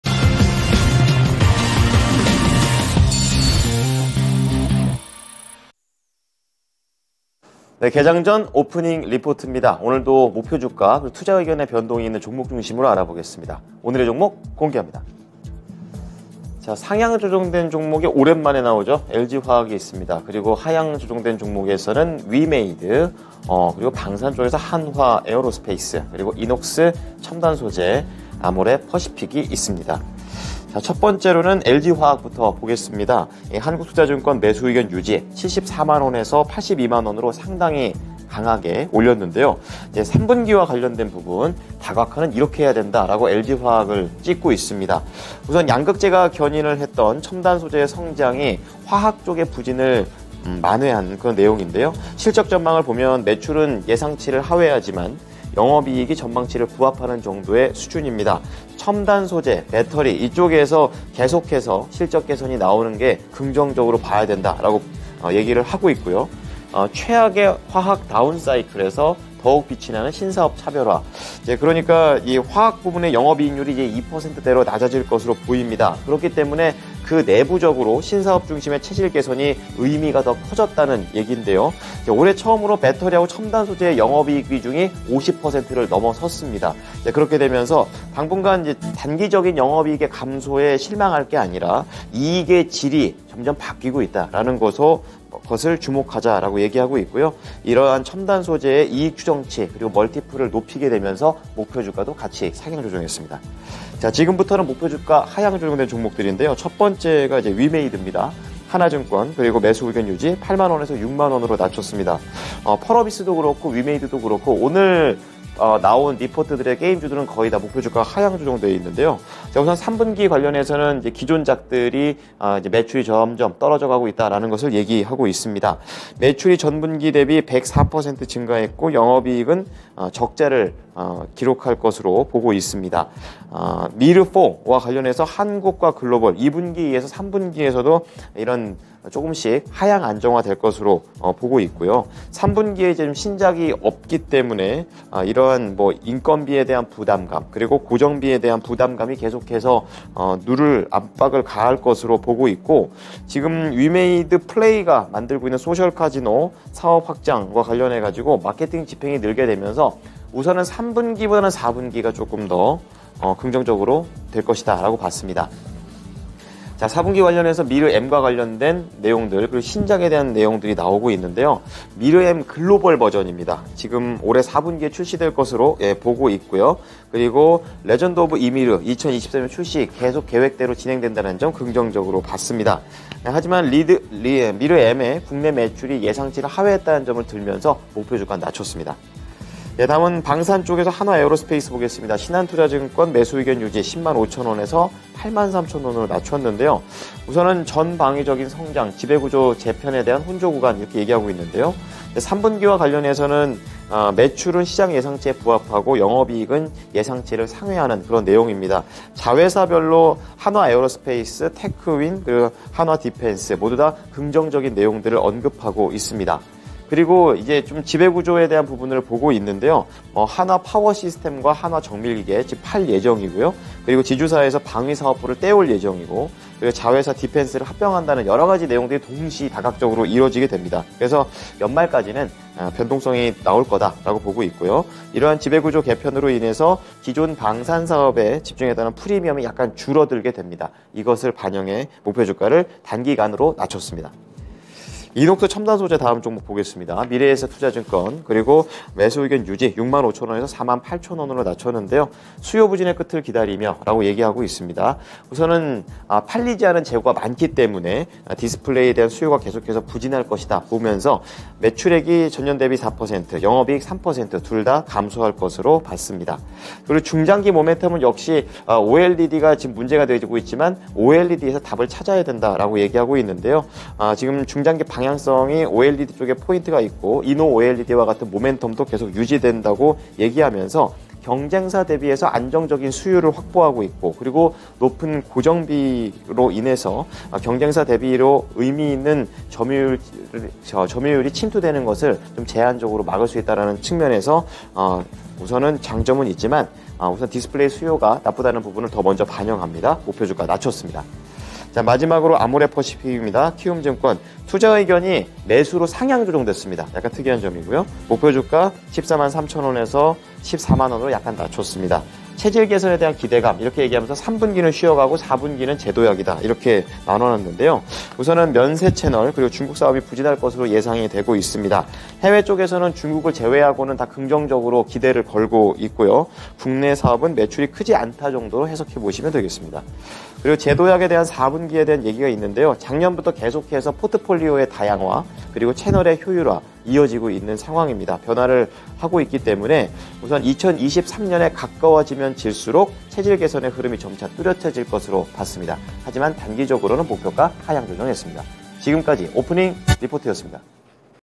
오프닝벨: 오늘장 주요 리포트 / 상향: LG화학 / 하향: 위메이드·한화에이로스페이스·이녹스첨단소·아모레퍼시픽
열린시장 오프닝 리포트
진행 : 이건
▶ 종목명: LG화학
증권사: 한국투자증권
투자의견: BUY(유지)
목표주가: 740,000원 → 820,000원(상향)
제목: 3Q22 Preview: 다각화는 이렇게 해야

- 양극재가 견인한 첨단소재의 성장이 화학 부진을 만회
- 최악의 화학 다운사이클에서 더욱 빛이 나는 신사업 차별화

▶ 종목명: 위메이드
증권사: 하나증권
목표주가: 80,000원 → 60,000원(하향)
제목: 게임 매출은 내년부터

- 3분기 Preview: 기존작은 하향 안정화 수순
- 4Q 미르M 글로벌, 23년 매드엔진 신작, 레전드 오브 이미르

▶ 종목명: 한화에어로스페이스
증권사: 신한투자증권
목표주가: 105,000원 → 83,000원(하향)
제목: 전방위적 성장, 지배구조 재편 혼조 구간

- 3Q22 Preview: 항공엔진, 디펜스, 전력장비 개선
- 연말까지 지배구조 재편소화 과정

▶ 종목명: 이녹스첨단소재
증권사: 미래에셋증권
목표주가: 65,000원 → 48,000원(하향)
제목: 수요 부진의 끝을 기다리며

- 높은 재고로 인해 디스플레이 수요부진
- 중장기 모멘텀은 역시 OLED

▶ 종목명: 아모레퍼시픽
증권사: 키움증권
투자의견: BUY(상향)
목표주가: 143,000원 → 140,000원(하향)
제목: 앞으로 체질개선 기대

- 쉬어가는 3분기
- 재도약의 4분기